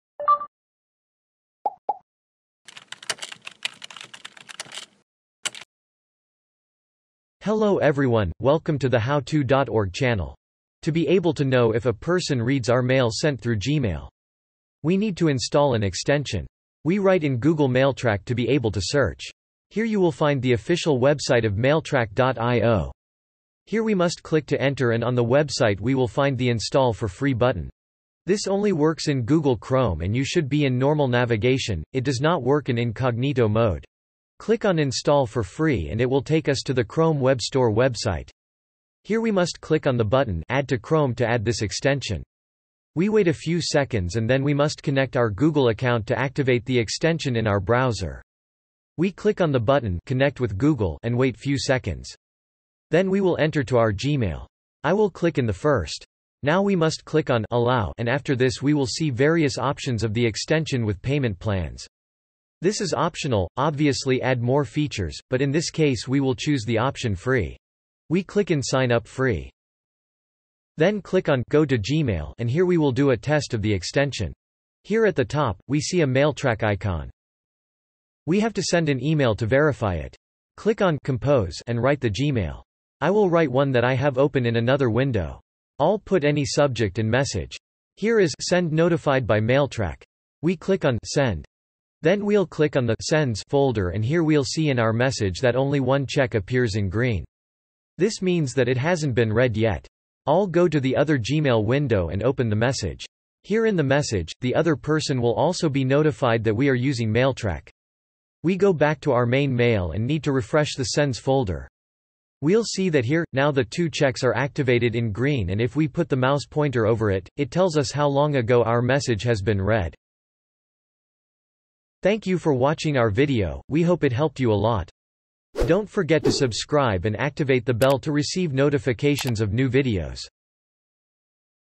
How to Know If Receiver Read Email Gmail | Trick with extension
Here, by installing a simple extension, you will be able to know if the recipient reads your email that you have sent. Like WhatsApp style, with blue tick marks.